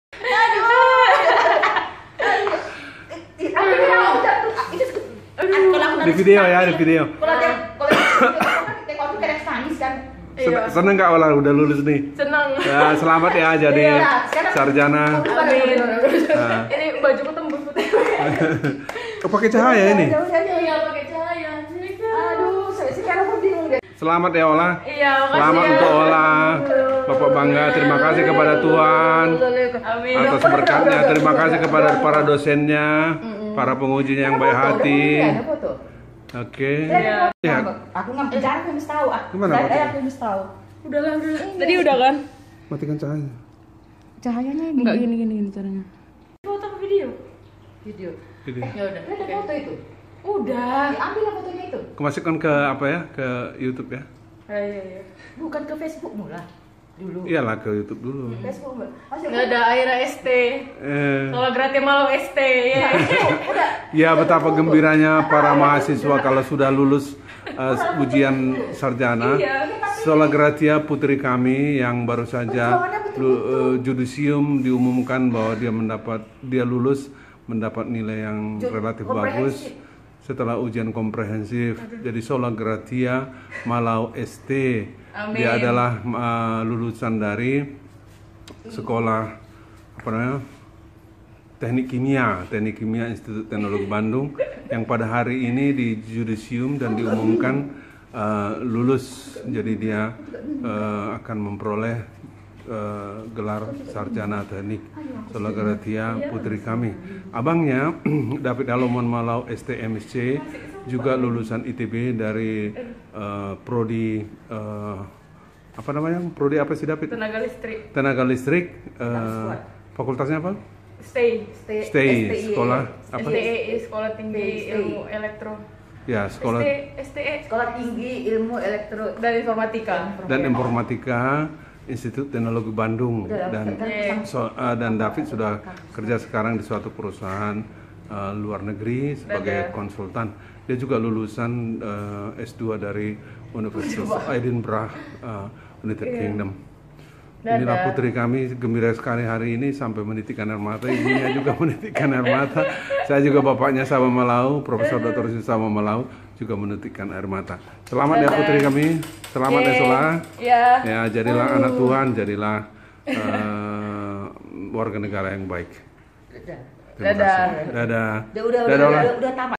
Momen sesaat setelah pengumuman kelulusan Sarjana ITB Sola Gratia Malau, ST
Momen sesaat pengumuman kelulusan Sarjana ITB Sola Gratia Malau, ST. Tanggal 23 Sept 2020.